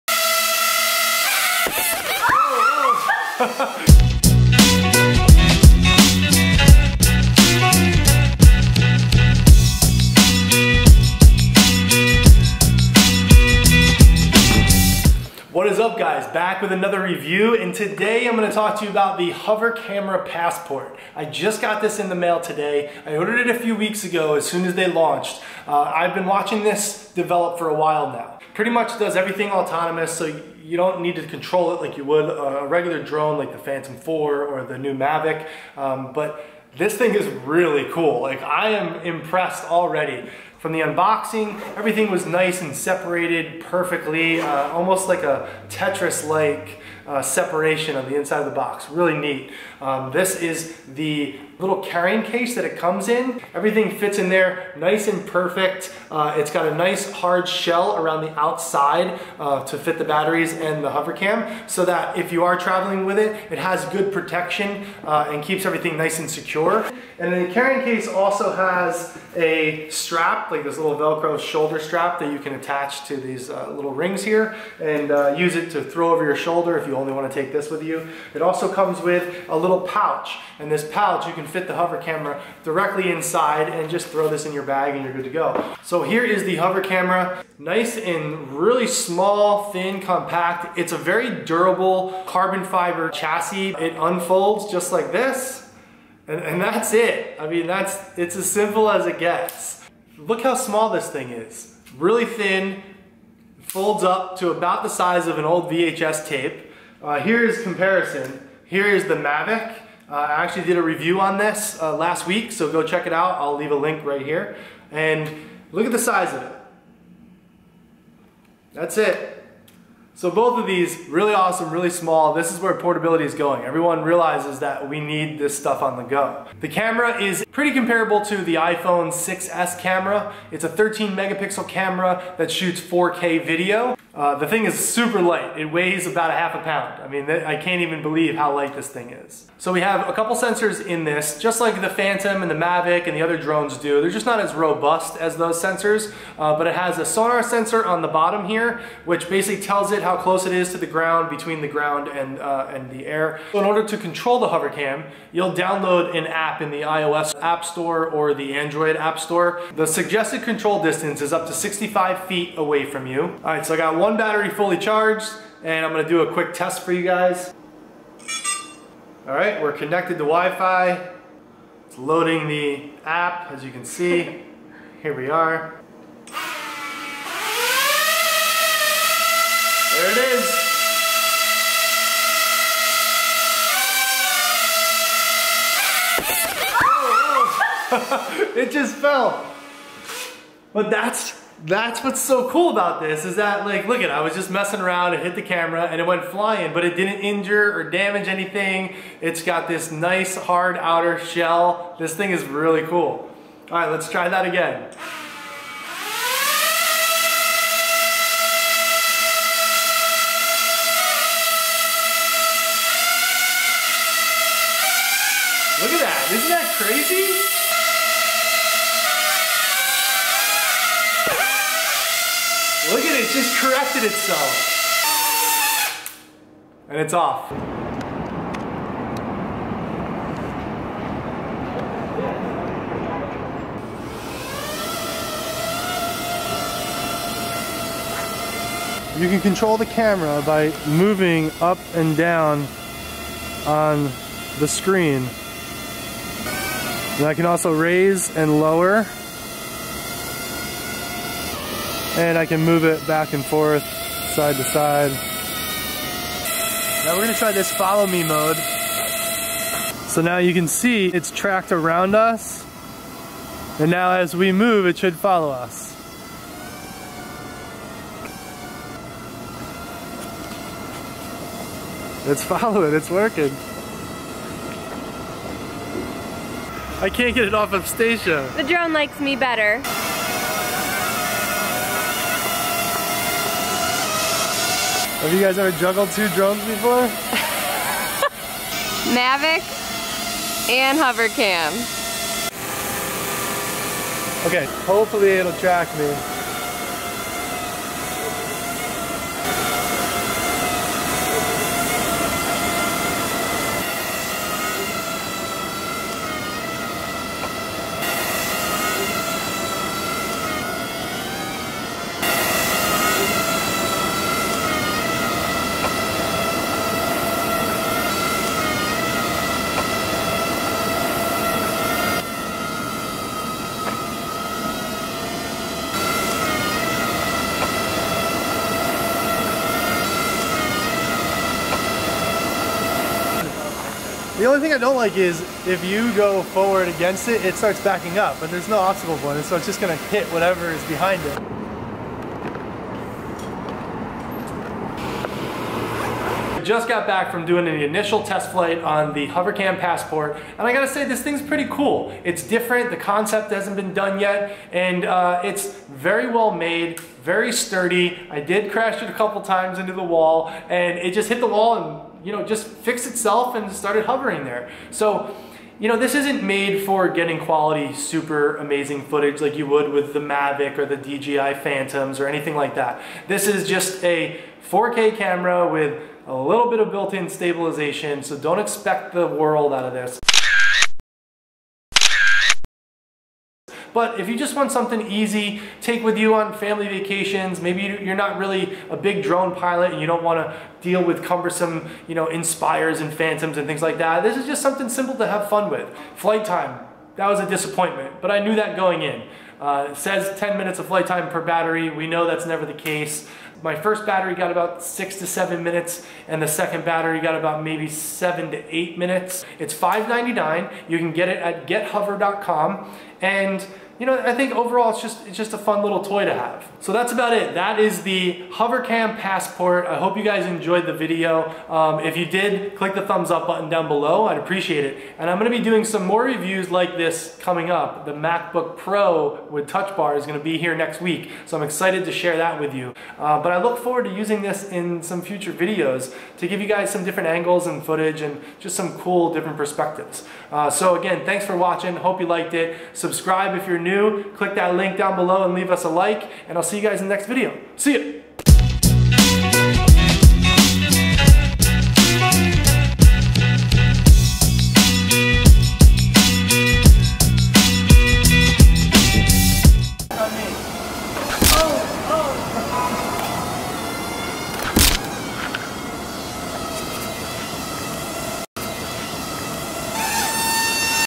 HOVER CAMERA REVIEW & UNBOXING
Hover Camera Review and Unboxing

Finally received the Hover Camera Passport to review. Very cool little gadget.

MY GEAR